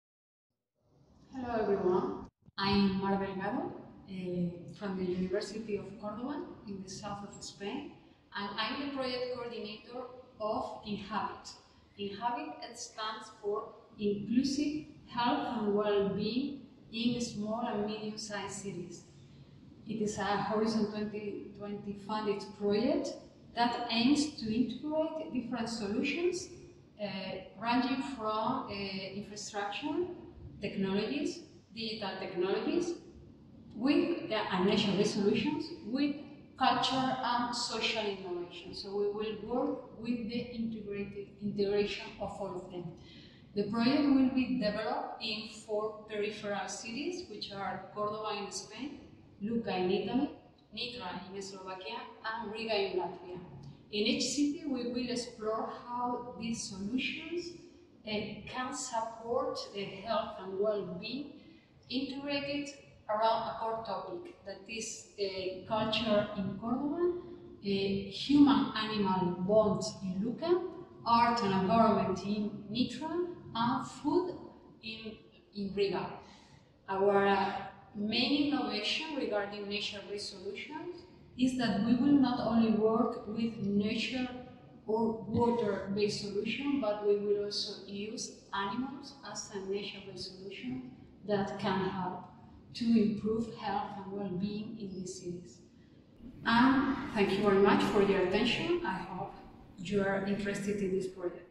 Introduction to IN-HABIT
INclusive Health And wellBeing In small and medium size ciTies (IN-HABIT) is an EU Horizon 2020 project that aims to foster Inclusive Health and Wellbeing in four peripheral small and medium size cities (SMSCs): Cordoba (Spain), Riga (Latvia), Lucca (Italy), and Nitra (Slovakia). In each city, the project will mobilise existing undervalued resources (culture, food, human-animal bonds and environment) to increase IHW, with a focus on gender and diversity. The integrated approach will combine technological, digital, nature-based, cultural, and social innovations in selected urban public spaces. These solutions will be co-designed, co-deployed and co-managed with and by local stakeholders.